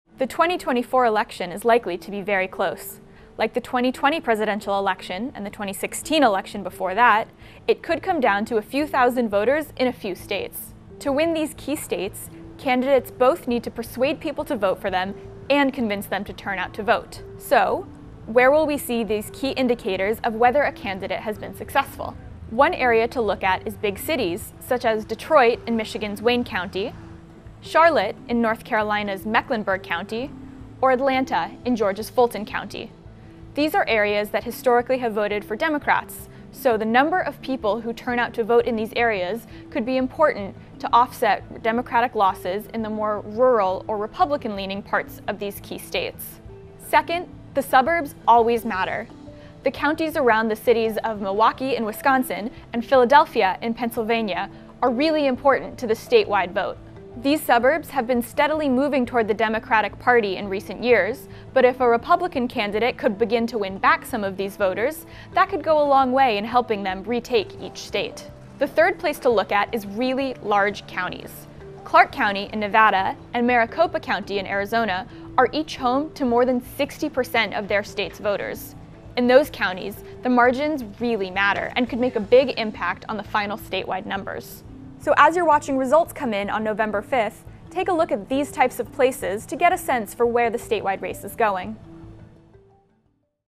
Explaining the Election: How thousands of voters in certain states might decide the election
RESTRICTION SUMMARY:

++MUSIC CLEARED FOR EDITORIAL USE++

ASSOCIATED PRESS
Washington - 1 October 2024
++PARTIALLY COVERED++
1. SOUNDBITE (English) Maya Sweedler, The Associated Press:
"The 2024 election is likely to be very close. Like the 2020 presidential election and the 2016 election before that, it could come down to a few thousand voters in a few states."

ASSOCIATED PRESS
ARCHIVE: Warren, Michigan – 27 February 2024
2. A worker sifts through a box filled with absentee ballots

ASSOCIATED PRESS
ARCHIVE: Lansing, Michigan - 28 October 2022
3. Mid of a sign reading (English) “Vote Here”

ASSOCIATED PRESS
++FULLY COVERED++
4. SOUNDBITE (English) Maya Sweedler, The Associated Press:
"To win these key states, candidates both need to persuade people to vote for them and convince them to turn out to vote. So where will we see these key indicators of whether a candidate has been successful?"

ASSOCIATED PRESS
ARCHIVE: Windsor, Ontario – 13 July 2024
5. The Detroit skyline as seen from across the Detroit River in Canada

ASSOCIATED PRESS
ARCHIVE: Detroit – 10 April 2024
6. Various of a “Detroit” sign installed along Interstate 94

ASSOCIATED PRESS
ARCHIVE: Matthews, North Carolina - 30 July 2024
7. Various of North Carolina flag in front of a library

ASSOCIATED PRESS
++FULLY COVERED++
8. SOUNDBITE (English) Maya Sweedler, The Associated Press:
"One area to look at is big cities such as Detroit in Michigan's Wayne County, Charlotte in North Carolina's Mecklenburg County or Atlanta in Georgia's Fulton County."

ASSOCIATED PRESS
ARCHIVE: Atlanta, Georgia - 9 June 2020
++MUTE++
9. Aerial of voters waiting in line to vote at Park Tavern
10. Tracking aerial of volunteers handing water to those waiting

ASSOCIATED PRESS
++PARTIALLY COVERED++
11. SOUNDBITE (English) Maya Sweedler, The Associated Press:
"These are areas that historically have voted for Democrats. So the number of people who turn out to vote in these areas could be important to offset Democratic losses in the more rural or Republican-leaning parts of these key states."

ASSOCIATED PRESS
ARCHIVE: Milwaukee - 28 June 2024
++MUTE AT SOURCE++
12. Drone aerial view of MIlwaukee skyline

ASSOCIATED PRESS
ARCHIVE: Milwaukee - 13 September 2024
13. Mid of polling signs leaning against wall

ASSOCIATED PRESS
++FULLY COVERED++
14. SOUNDBITE (English) Maya Sweedler, The Associated Press:
"Second, the suburbs always matter. The counties around the cities of Milwaukee in Wisconsin and Philadelphia in Pennsylvania are really important to the statewide vote."

ASSOCIATED PRESS
ARCHIVE: Bristol, Pennsylvania – 11 September 2024
15. Mural art in downtown
16. A group of people sitting outside a restaurant
17. Mid of Burlington Island in Delaware River

ASSOCIATED PRESS
++PARTIALLY COVERED++
18. SOUNDBITE (English) Maya Sweedler, The Associated Press:
"These suburbs have been steadily moving toward the Democratic Party in recent years. But if a Republican candidate could begin to win back some of these voters, that could go a long way in helping them retake each state."

ASSOCIATED PRESS
ARCHIVE: Las Vegas - 9 November 2022
19. Various election workers using machines to count ballots

ASSOCIATED PRESS
ARCHIVE: Mesa, Arizona - 4 March 2024
20. Woman dropping off ballot in Maricopa County ballot drop box

ASSOCIATED PRESS
ARCHIVE: Phoenix - 5 March 2024
22. Close-up of an election official flipping through mail ballots